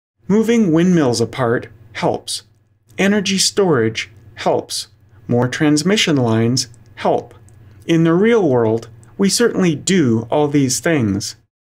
4h48m09s18f Move Windmills Apart - Energy Storage - Transmission Lines - TR2016a
Moving the windmills apart helps. Energy storage helps. More transmission lines help. In the real world, we certainly do all these things. -Gordon McDowell